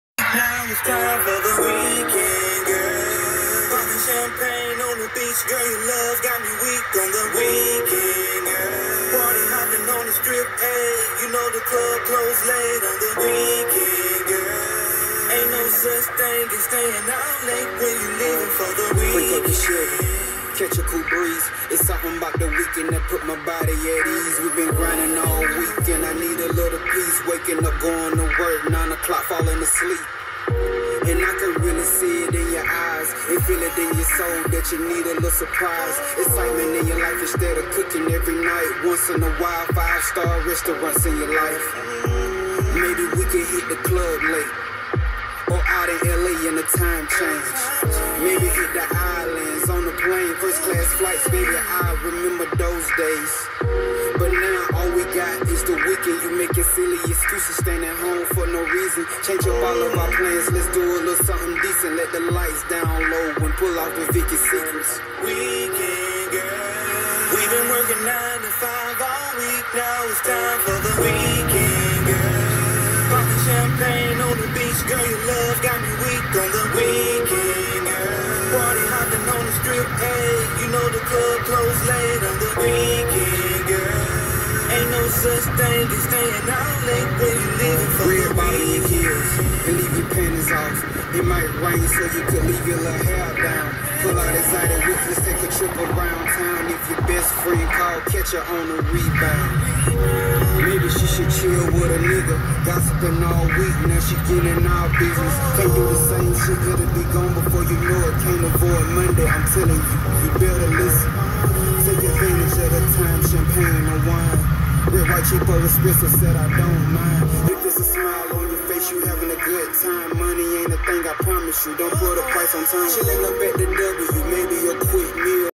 NFL 25 PRO RIDERS- Music by: K. Harris / 1957 Bel air Convertible ls3 swap, Budnik wheels, Wilwood
Ghost 57 Bel air Convertible ls3 swap, 525hp, Budnik wheels, Wilwood brakes